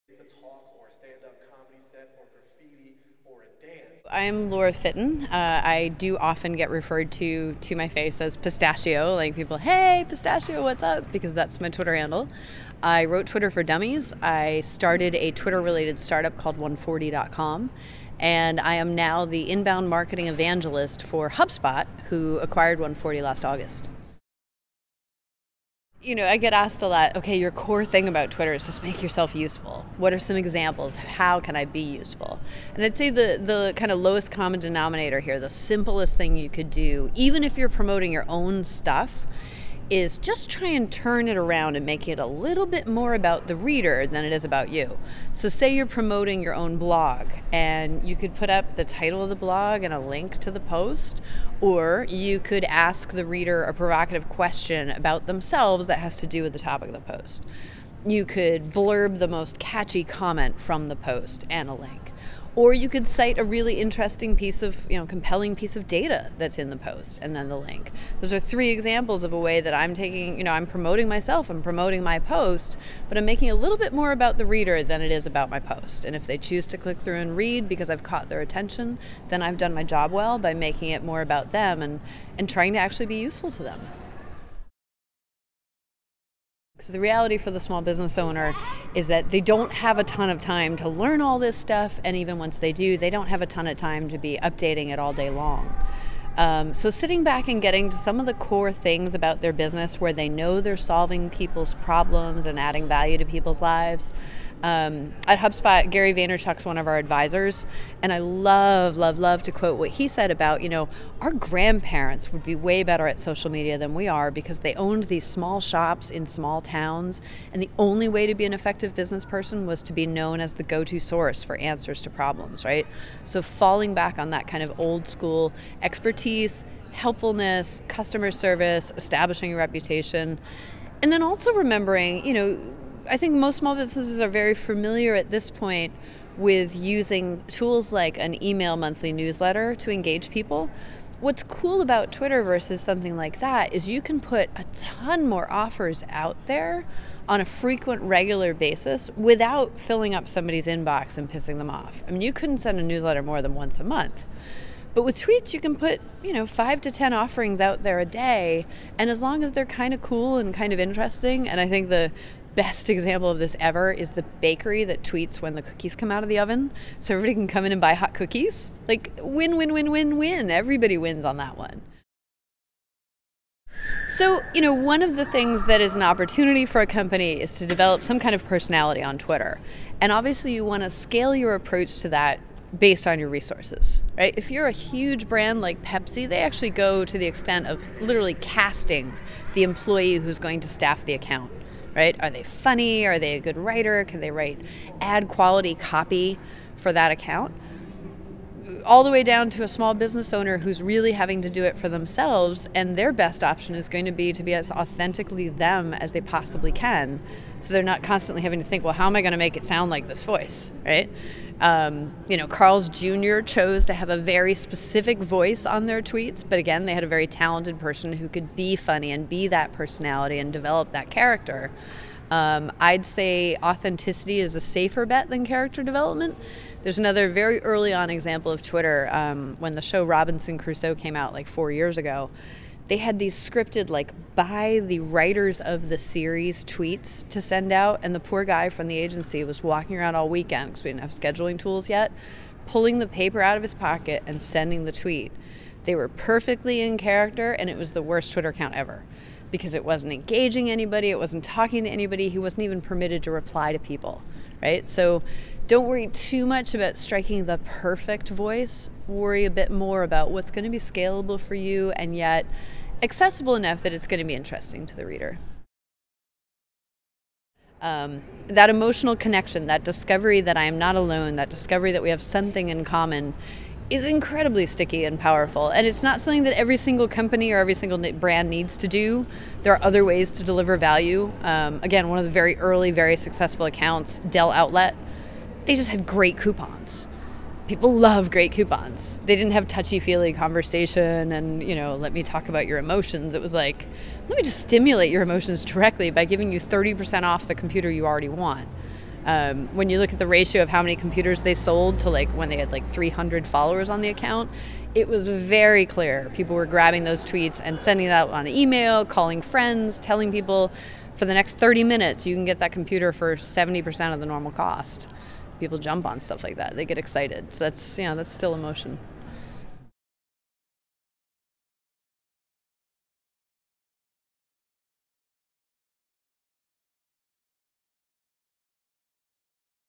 Laura Fitton talks about how to be useful on Twitter, building an audience, and warm cookies
Laura Fitton is a Twitter evangelist and currently works at HubSpot. She is also the author of Twitter for Dummies.

Recorded at WebVisions PDX 2012